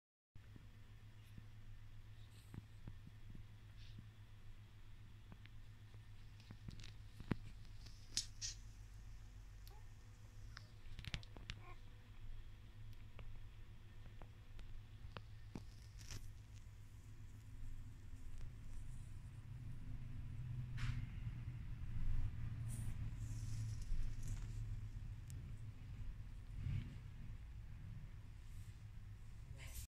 Whippet puppies
Seven little pups, 4 days old.